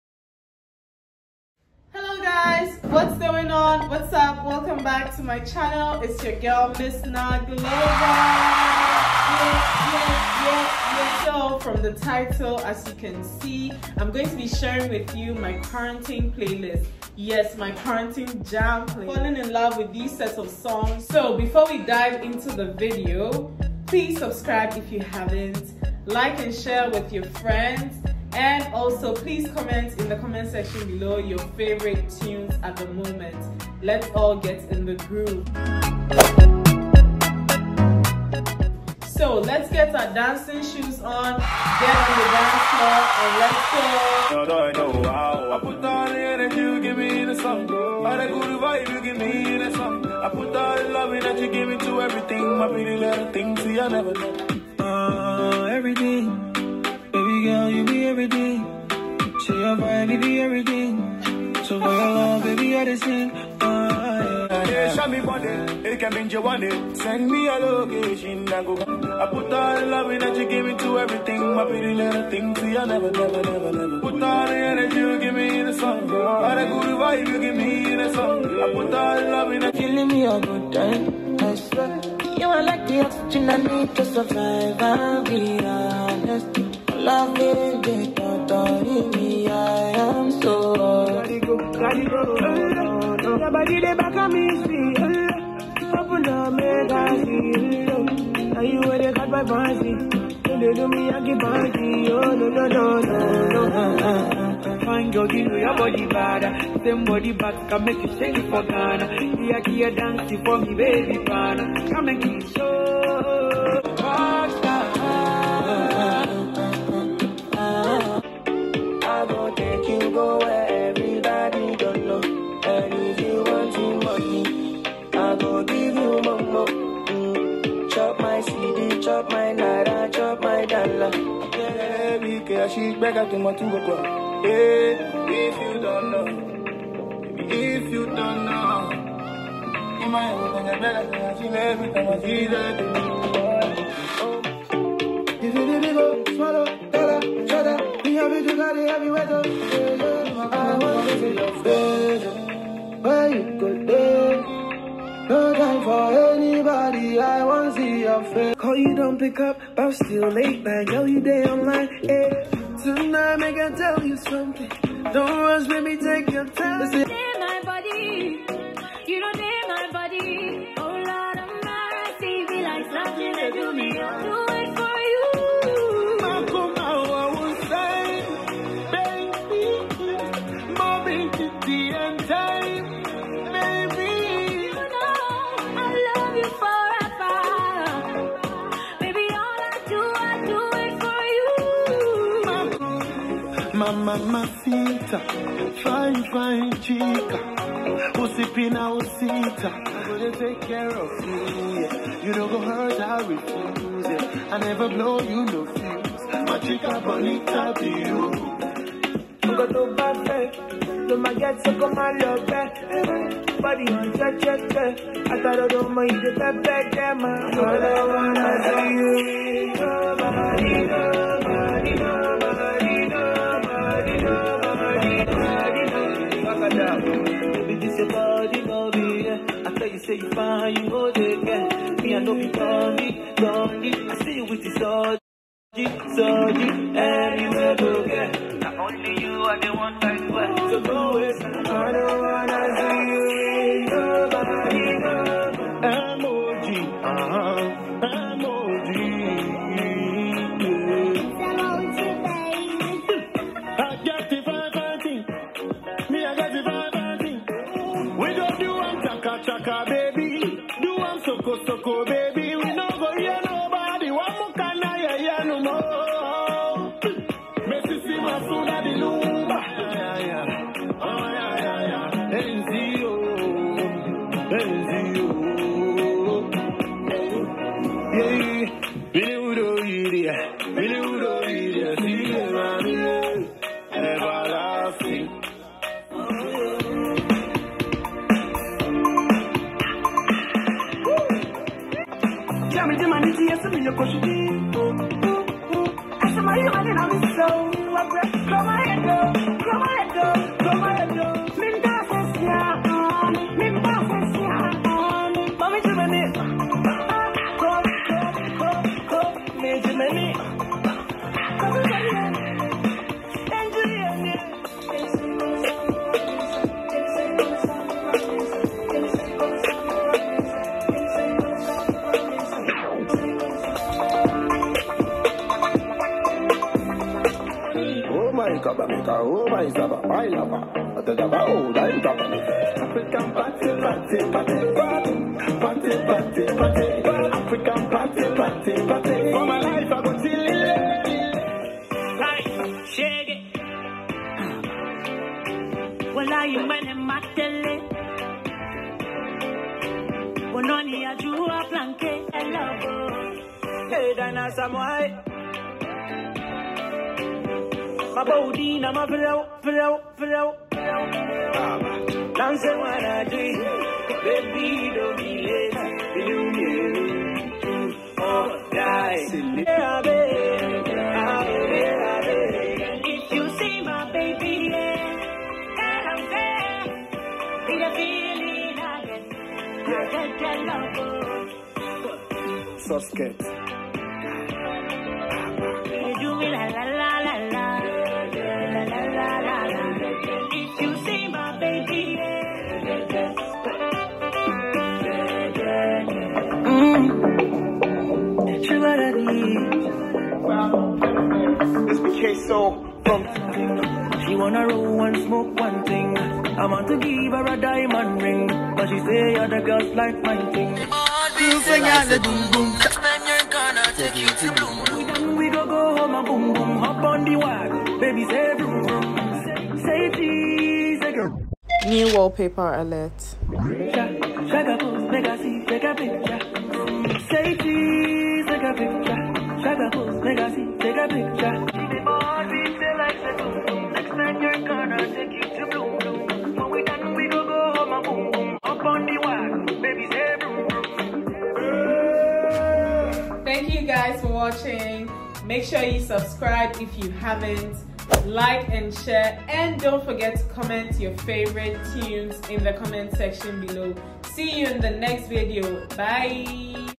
My Quarantine Playlist (Ghana version 🇬🇭)
A compilation of Ghanaian and Nigerian songs I have fallen in love with while working from home. Enjoy!!